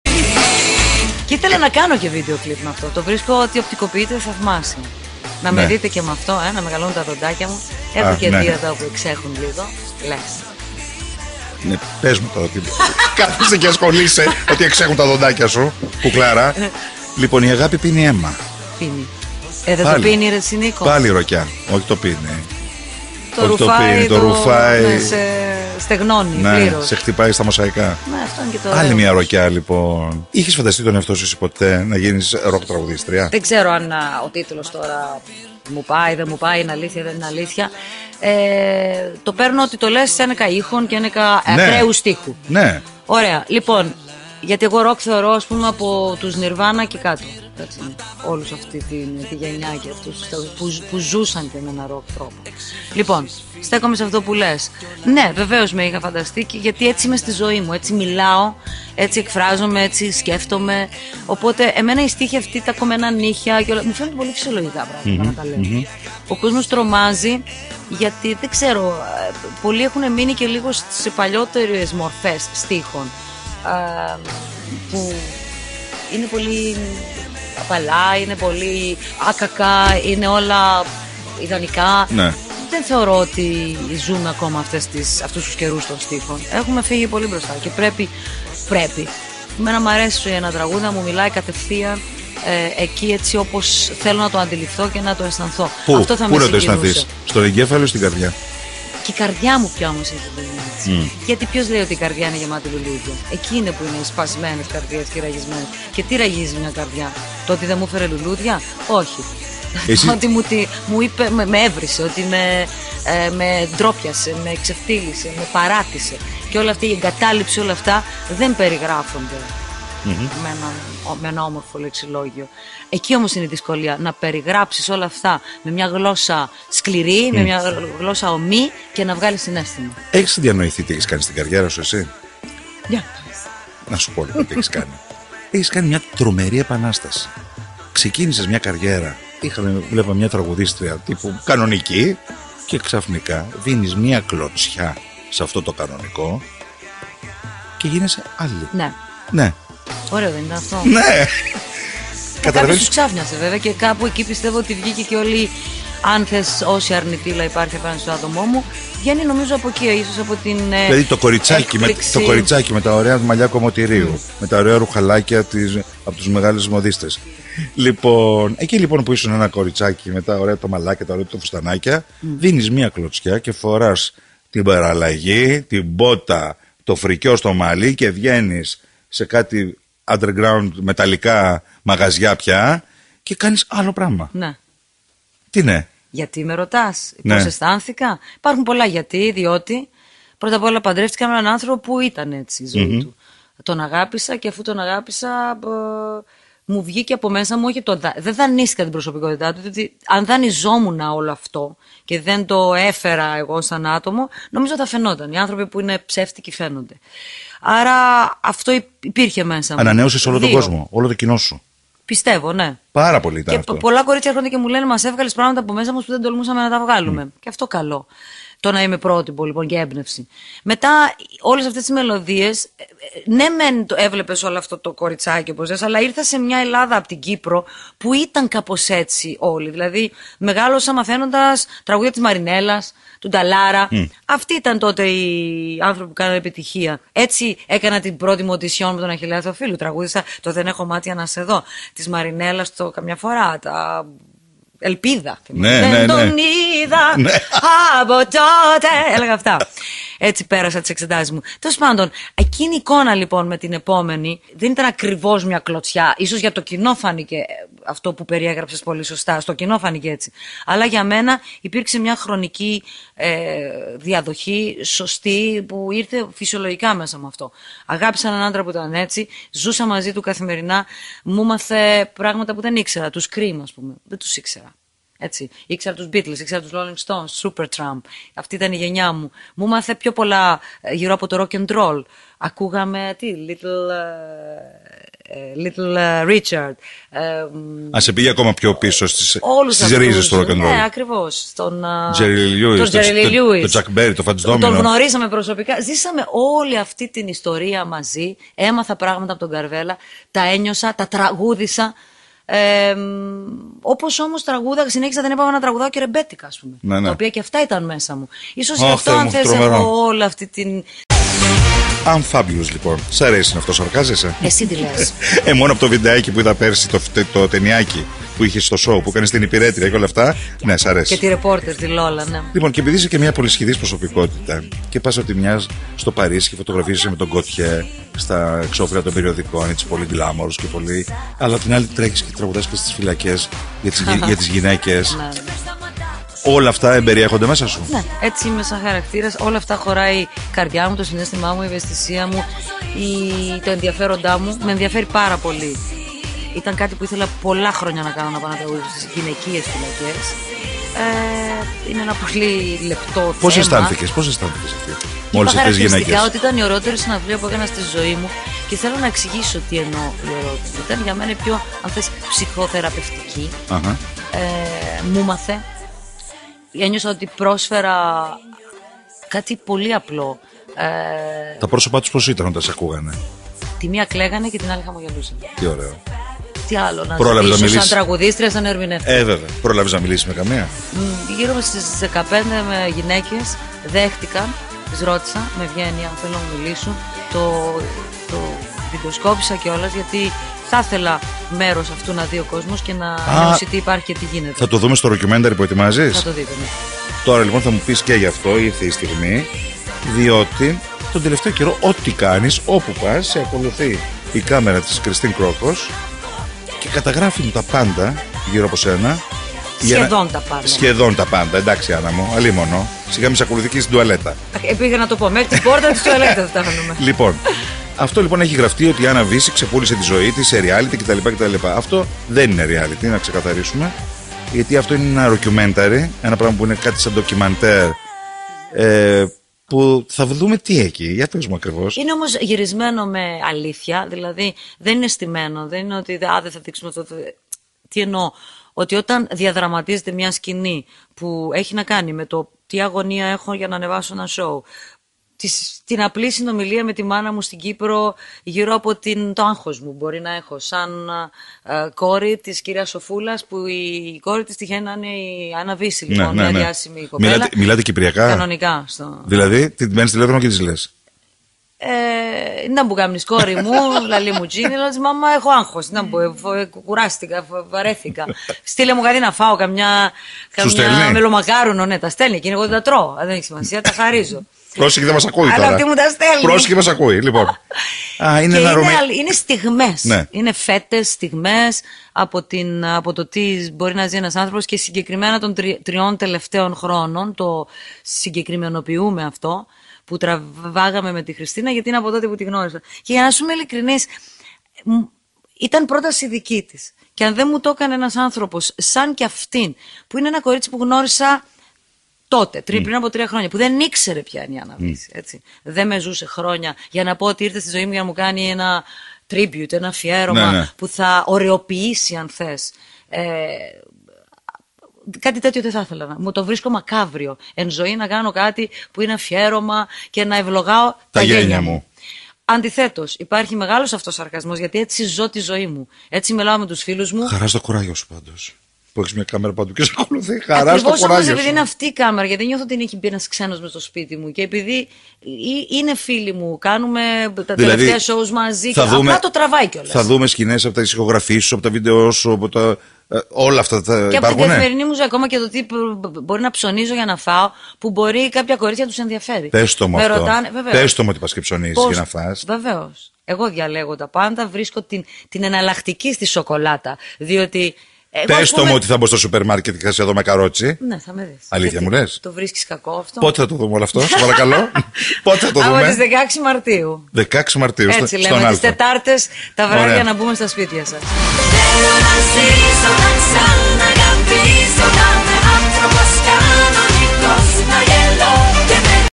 Η Άννα Βίσση μιλάει στο Νίκο Μουρατίδη (08/03/2011) [4ο Μέρος]
Anna Vissi talks to Nikos Mouratidis (08/03/2011) [Part 4]